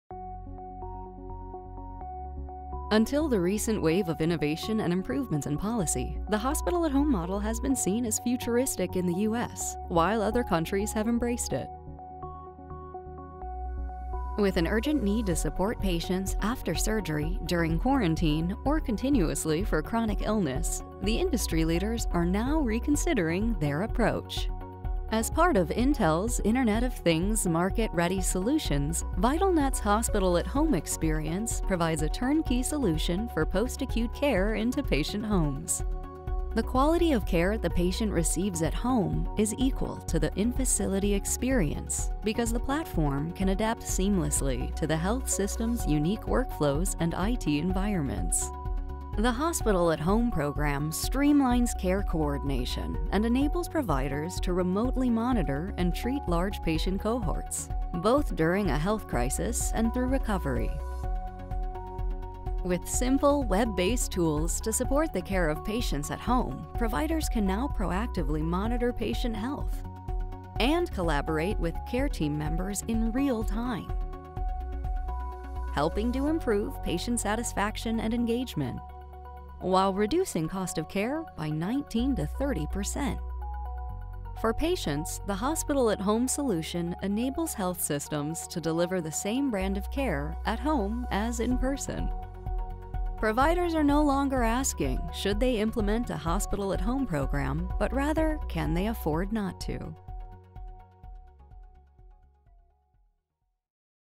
ViTel Net and Intel Deliver Hospital at Home
Pioneers ViTel Net and Intel have partnered to deliver a robust Hospital At Home solution that brings quality care directly to the patient home. This new model has been proven to reduce costs by 19-30% while improving patient outcomes reducing readmissions.